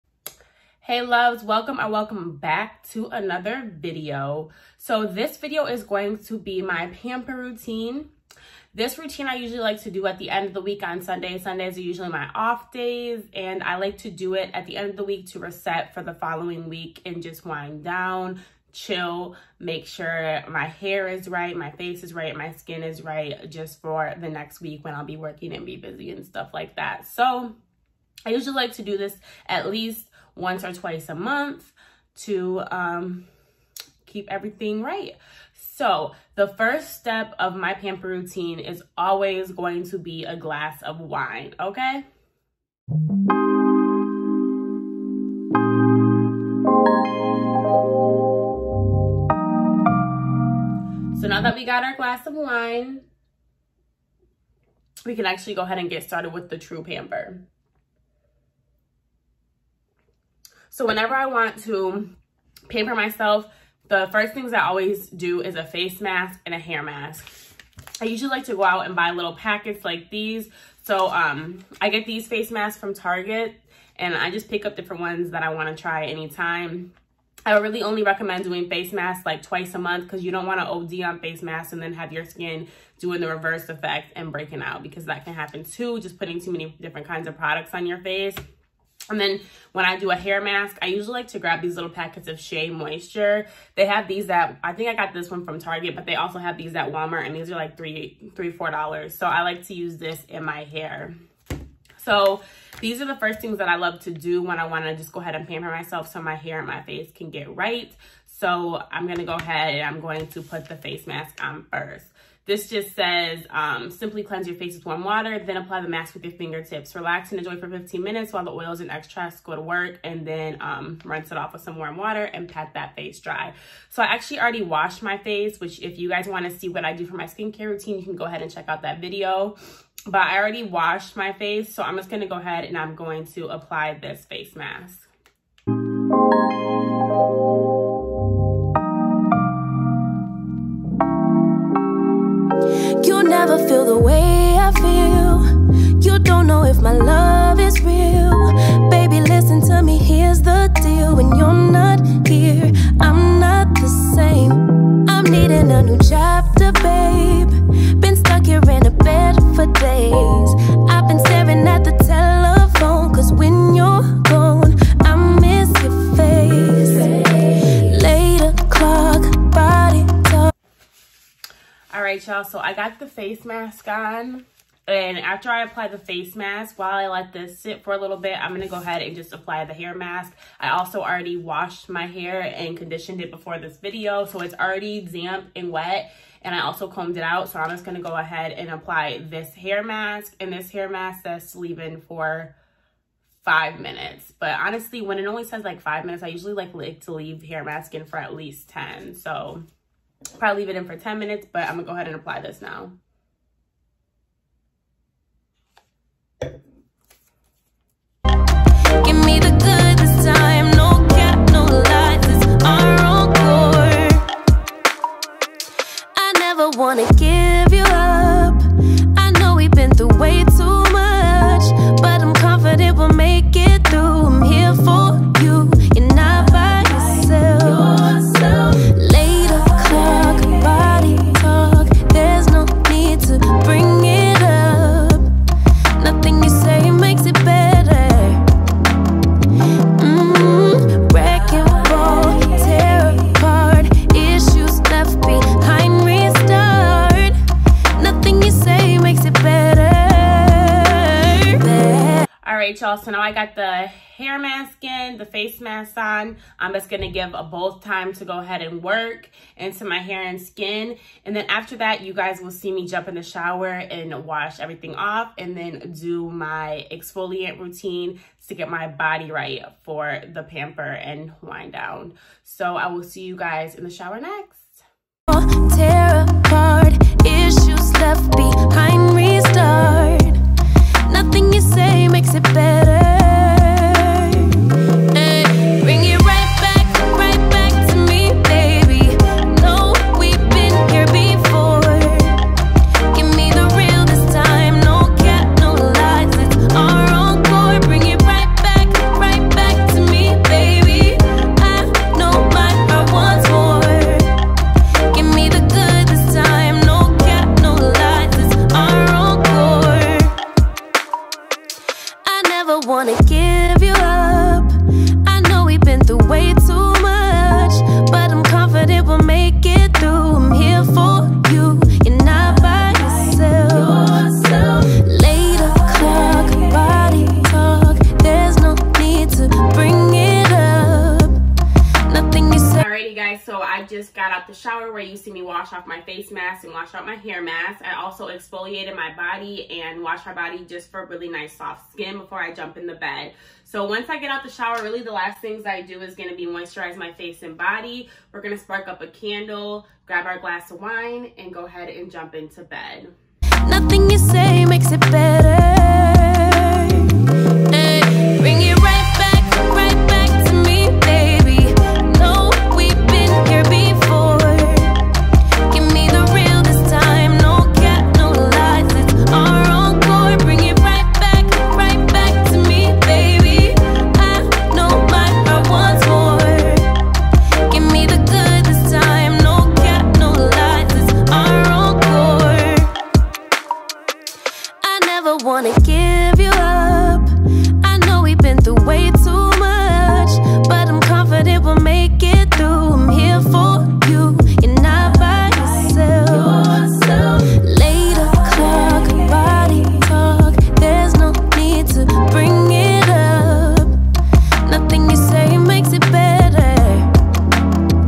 QUICK AND EASY PAMPER ROUTINE!
Hey loves, welcome or welcome back to another video. In this video I show you guys my quick pamper routine I do when I want to wind down and get ready for my upcoming week. I hope you guys enjoy!
Until next video, Mwuahh!